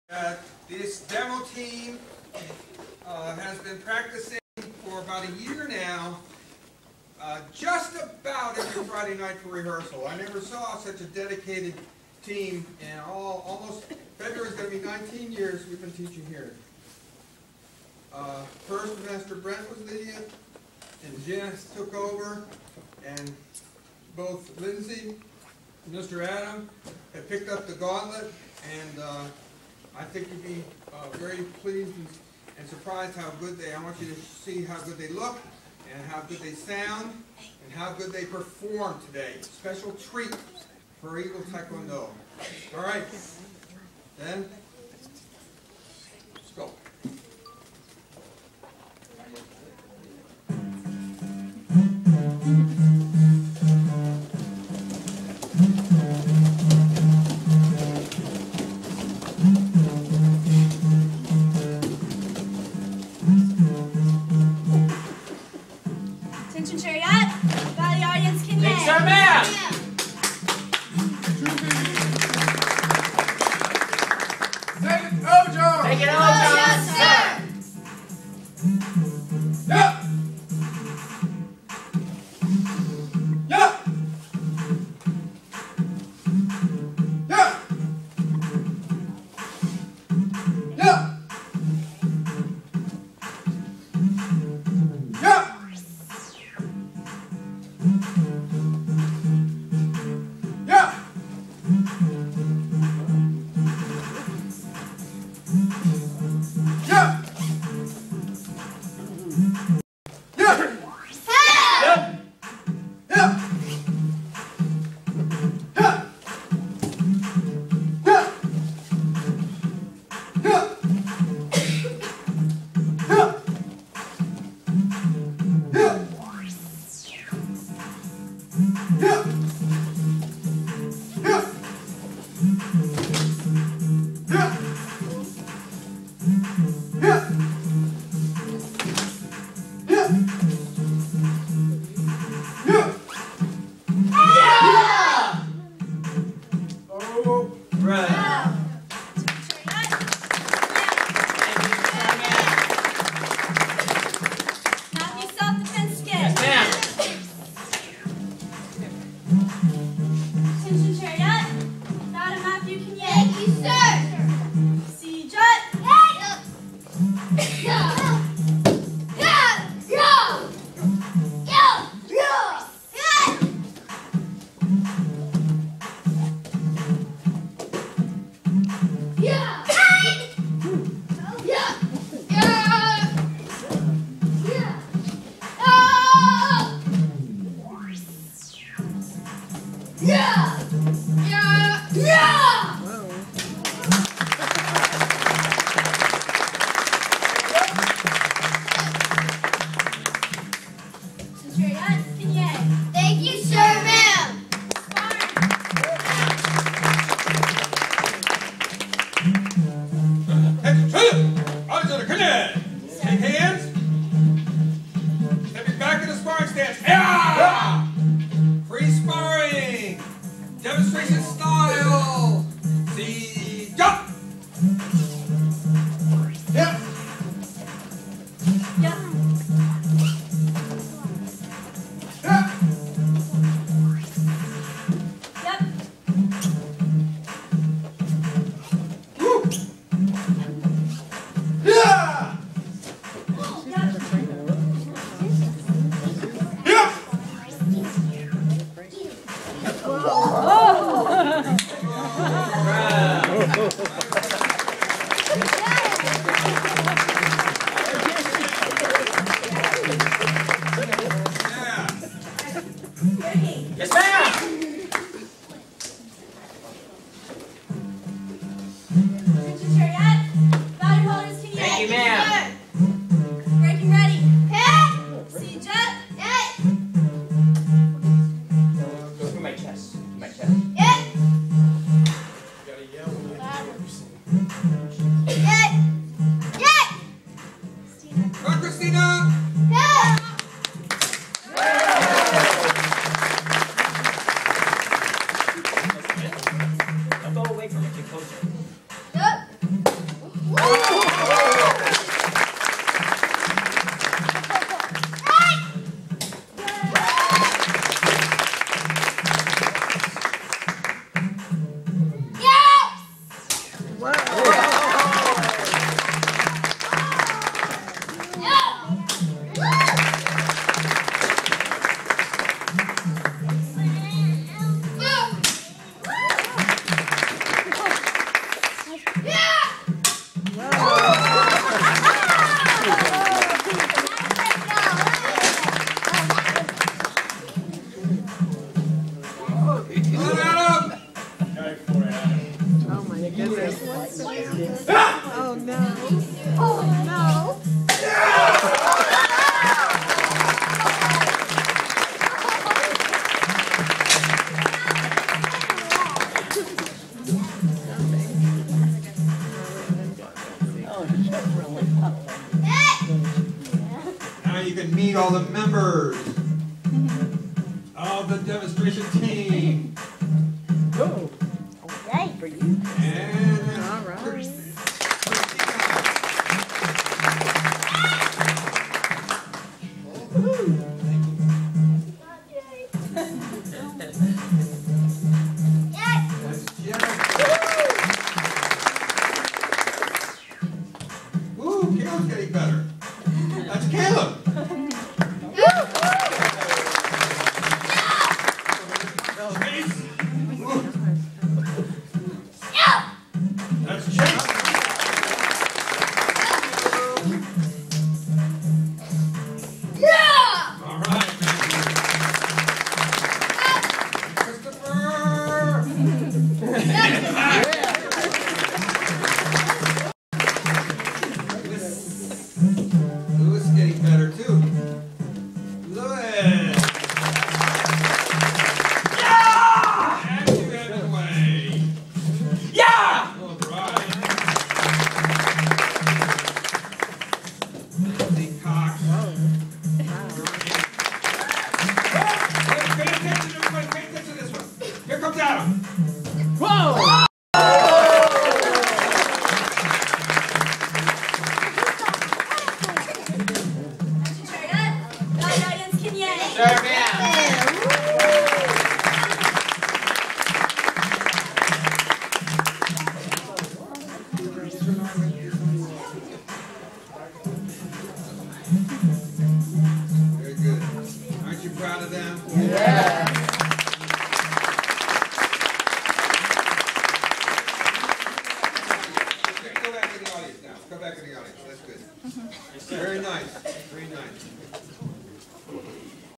2010-12-04 ETF Demo Team - Performance2
The ETF Demo Team performed at the beginning of the second belt promotion. Master Grady was a bit more into it, and they were less nervous (our girl still had trouble breaking her board, but I later found out she wasn't feeling well).  GO ETF! :)